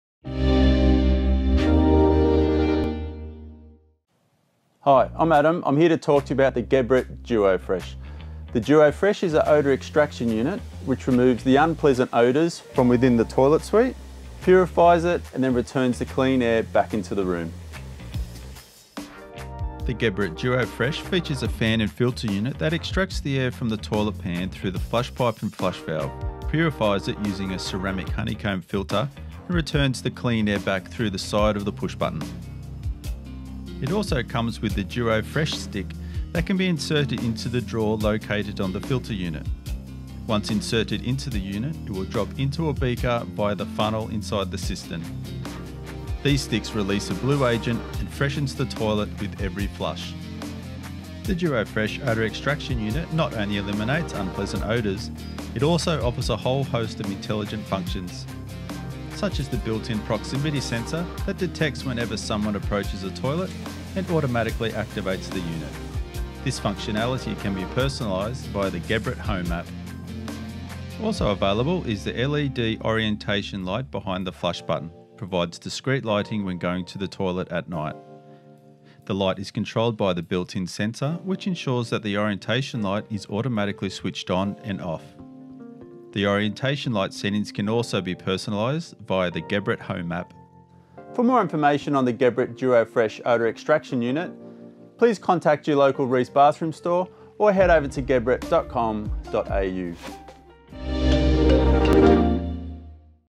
Geberit DuoFresh Functionality
The Geberit odour extraction unit removes unpleasant odours directly from the toilet. It extracts the air through the flush pipe and flush valve, purifies it using a ceramic honeycomb filter, and then returns it to the room.